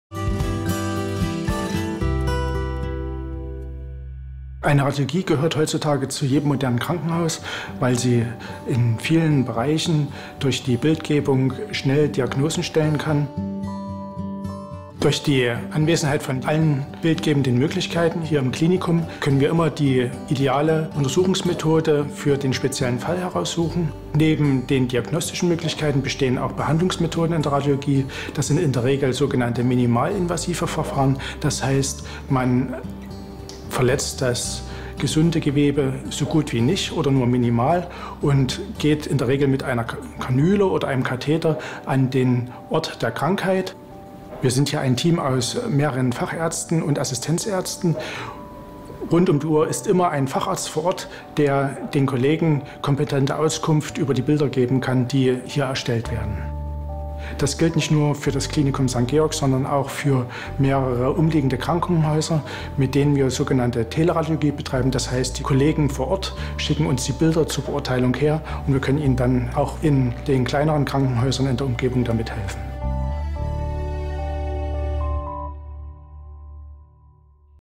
Radiologie in Leipzig, Diagnostik und Therapie | St. Georg TV
Die Klinik für Diagnostische und Interventionelle Radiologie ist am Standort Eutritzsch und am Standort Grünau des Klinikums St. Georg in Leipzig vertreten. Die Klinik bietet das gesamte Spektrum der modernen bildgebenden Verfahren und minimalinvasiven Therapien an.